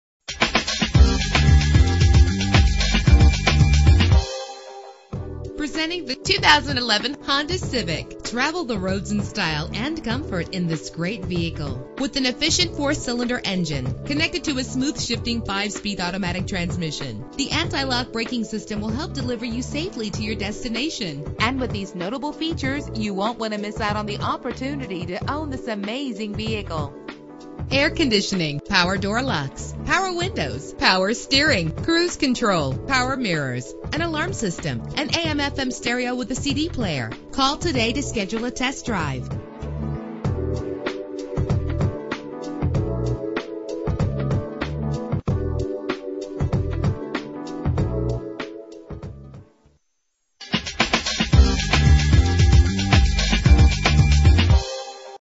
2011 Honda Civic - Denison TX
Year: 2011
Make: Honda
Model: Civic
Trim: LX
Engine: 1.8 liter inline 4 cylinder 16 valve
Transmission: 5-Speed Automatic
Color: Taffeta White
Mileage: 15
Address:
2020 U.S. Hwy 75 N.